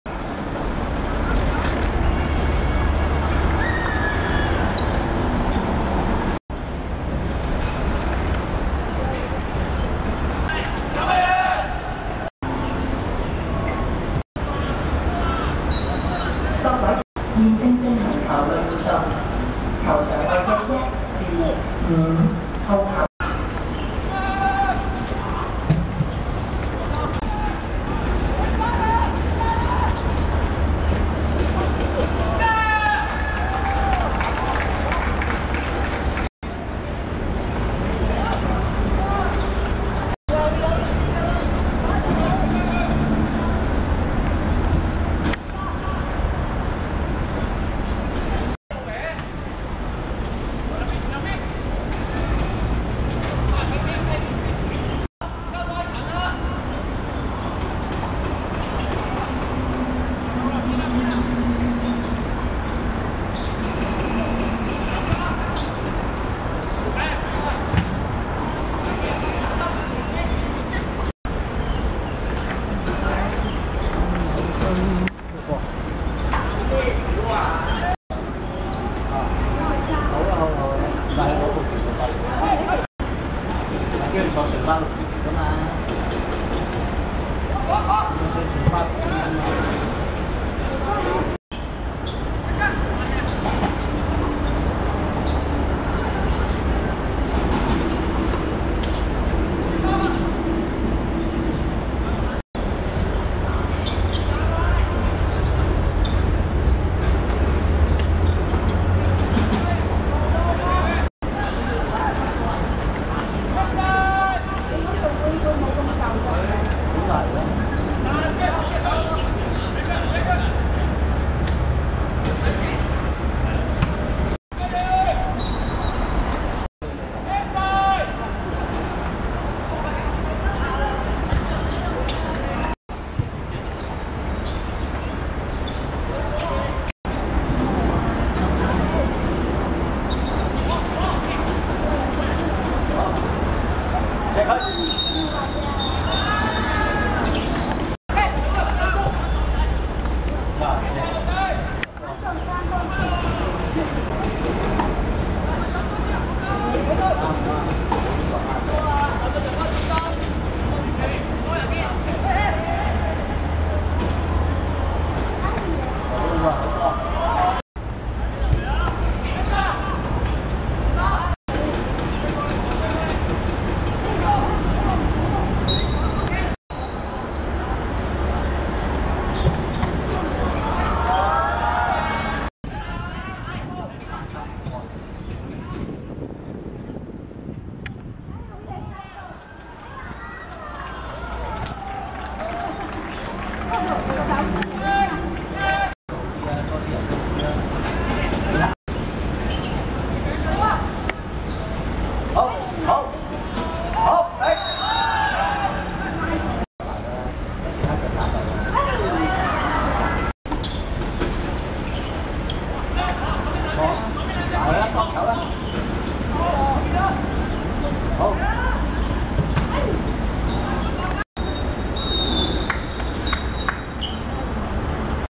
譚伯羽vs邱子田(2014.11.1.屯門學界足球甲組十六強)精華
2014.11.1.屯門鄧肇堅運動塲 9:30 譚伯羽(白) 2:0 邱子田(藍)